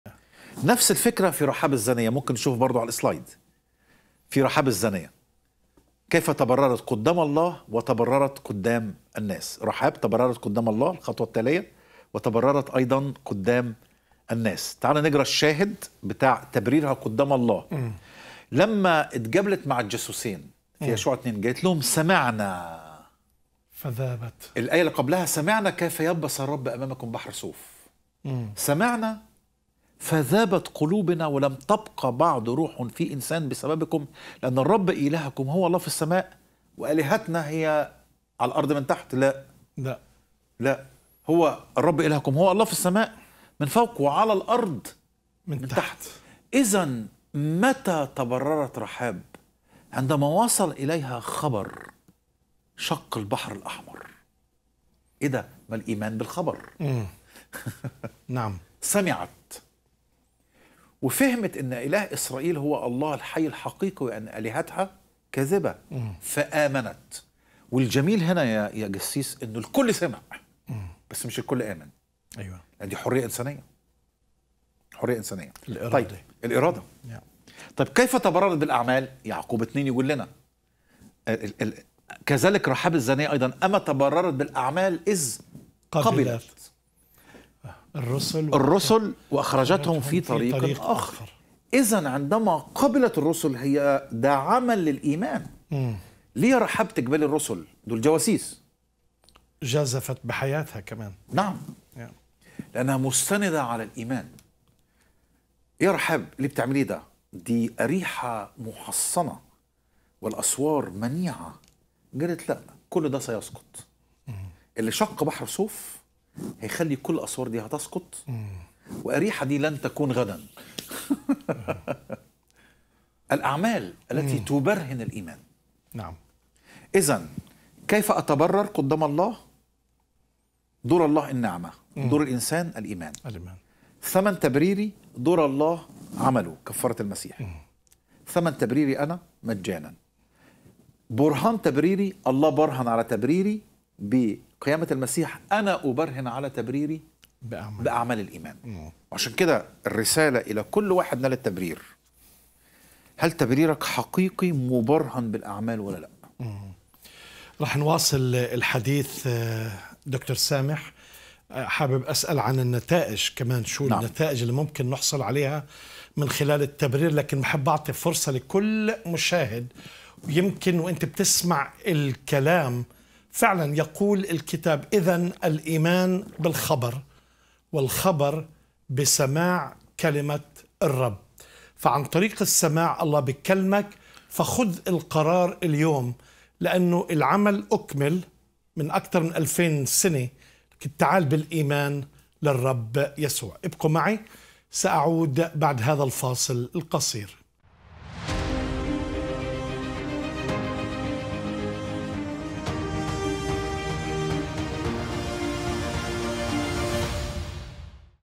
632 التبرير في حياة راحاب الزانية
التبرير في حياة راحاب الزانية
مقطع من الحلقة 632 – أنواع التبرير
لمشاهدة الحلقة كاملة تفضل بالضغط على هذا الرابط: